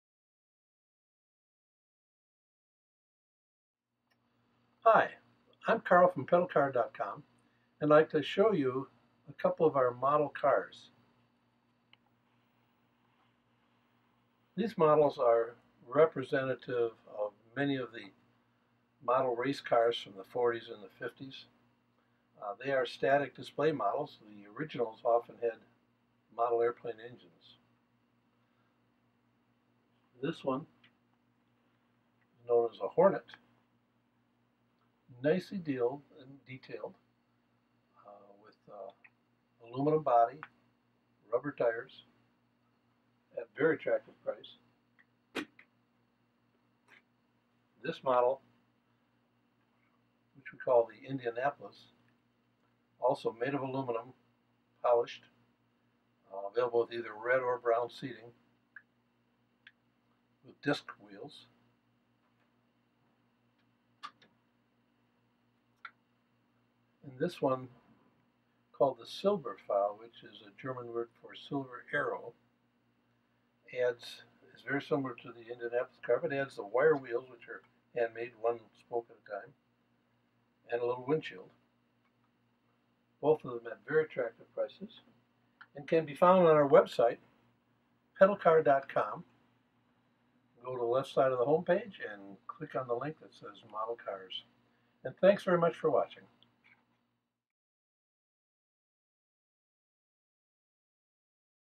Aluminum Model Cars From 30s, 40s and 50s | Pedalcar.com
Grab these amazingly detailed aluminum model cars with handcrafted parts and high-quality attention to detail. If you are a serious collector, or just a model enthusiast you'll love these attractively priced replicas of the Hornet Toy Speed Car, the Indianapolis 1930s Tether Aluminum Replica Racecar, and the classic Silberpfeil Indy Racecar models. This video shows all moving parts, the high-quality construction and close up shots of PedalCar.com's mini model cars that are a collector's dream. or talk to the man in the video, Carl Kriewall, himself at .